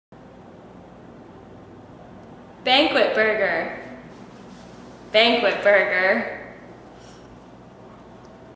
How to say words - banquet burger
banquet burger - Cdn a hamburger with bacon and cheese